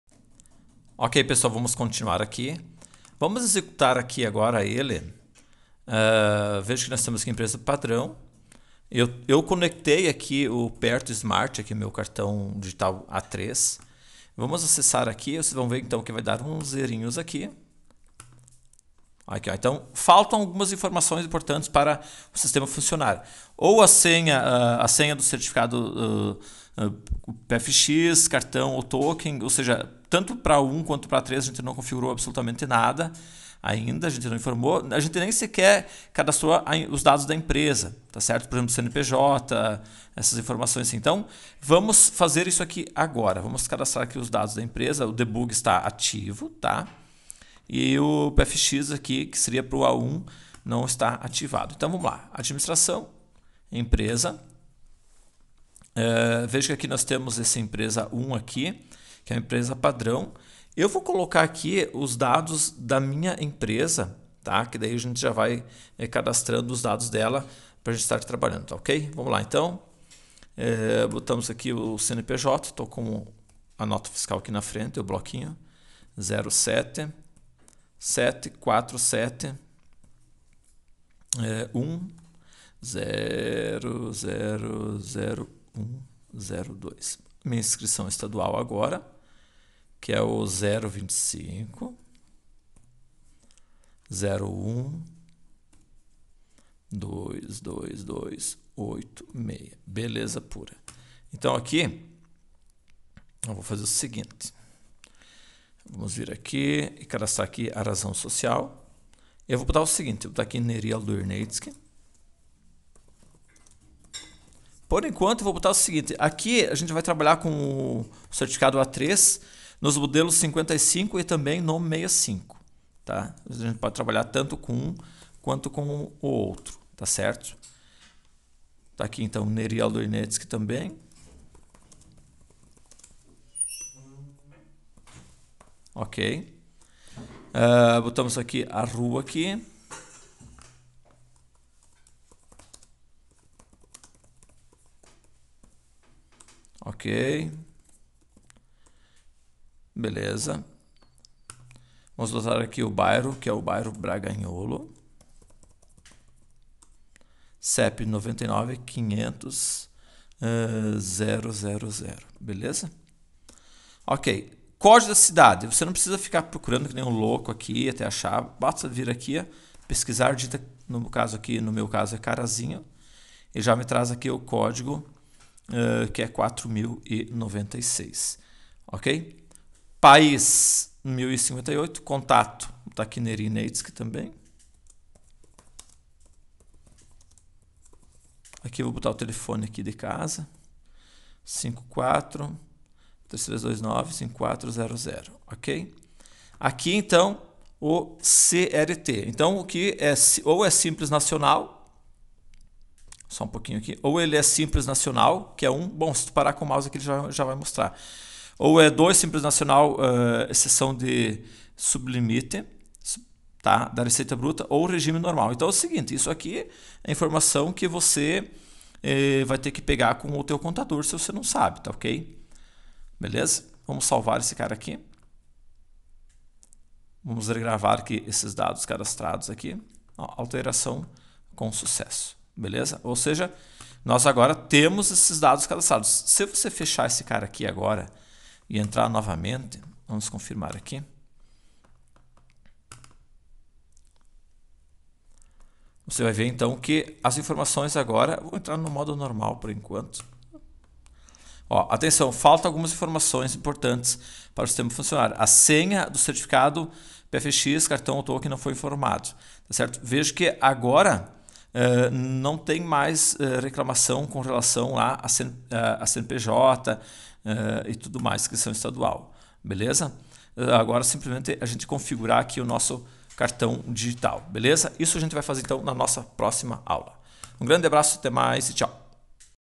aula 7105 NFe4 0 Cadastrando Empresa Emitente para A1 e A3
Bem vindos ao curso de Sistema de NFe NFce Nota Fiscal Eletrônica 4.0, modelos 55 e 65, com certificados digitais A1 e A3, vejam a descrição completo desse curso no